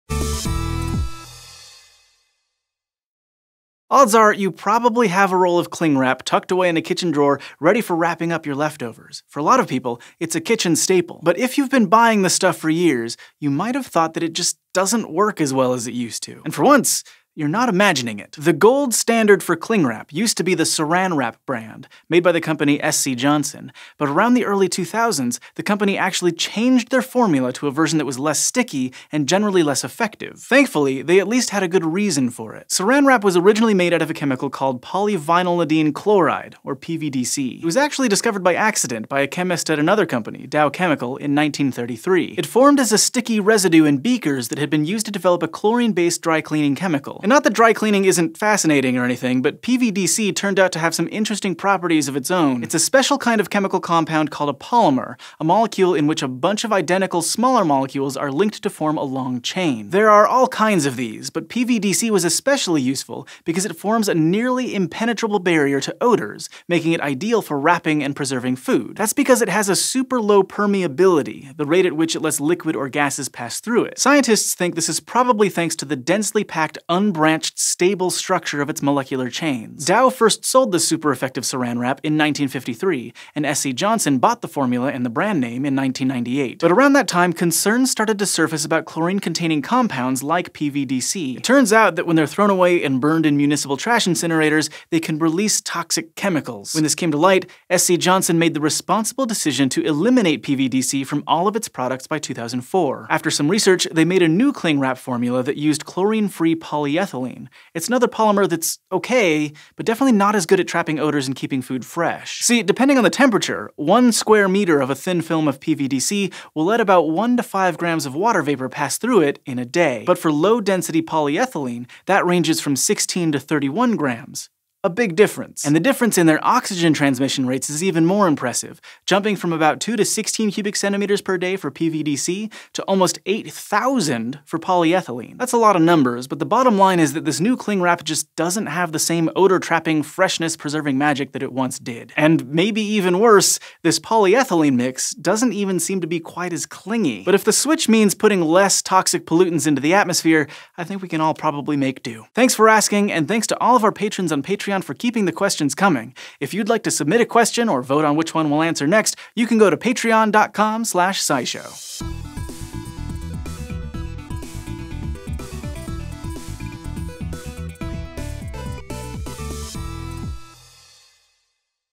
Why Isn't Cling Wrap as Good as It Used to Be?
"Back in my day, cling wrap was so much better!" Have you ever wondered why cling wrap doesn't seem to work as well as you remember it to?